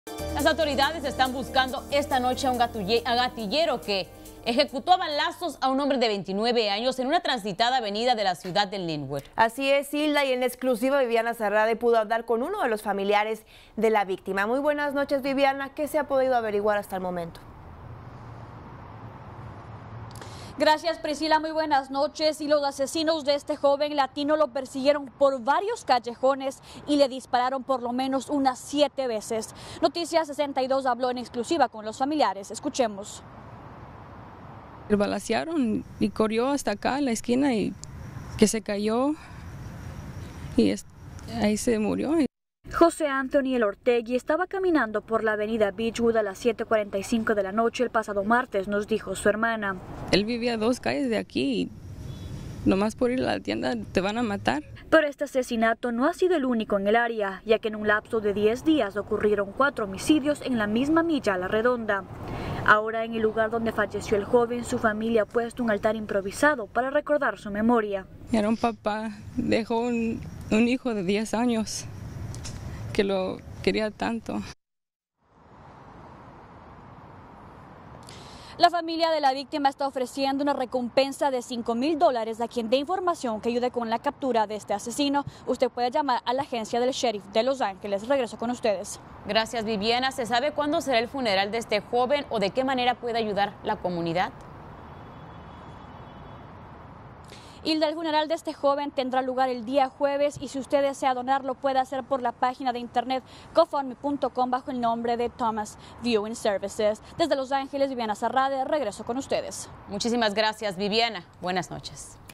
GATILLERO QUE EJECUTO A BALAZOS-Noticias 62
LAS AUTORIDADES ESTAN BUSCANDO ESTA NOCHE A UN GATILLERO QUE EJECUTO A BALAZOS A UN HOMBRE DE 29 AÑOS EN UNA TRANSITADA AVENIDA DE LA CIUDAD DE LYNWOOD.